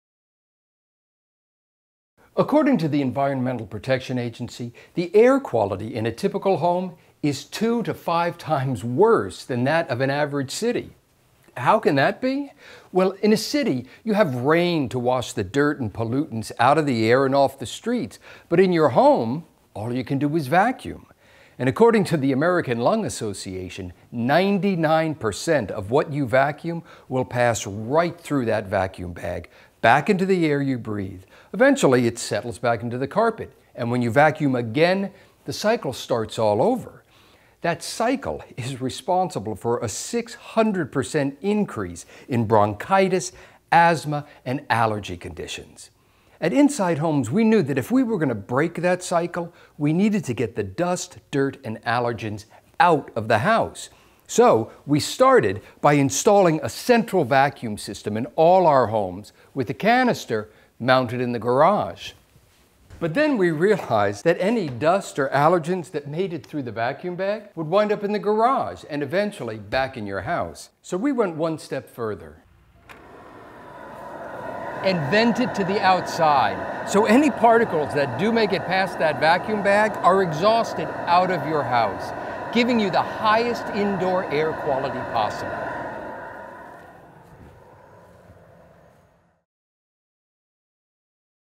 Indoor Air Quality: Central Vacuum
Poor indoor air quality leads to a variety of serious health issues. These series of videos will show how we drastically improved the indoor air quality in the new homes in Delaware, by using low VOC paints and carpets, superior air filtration, and central vacuum system that goes one step further.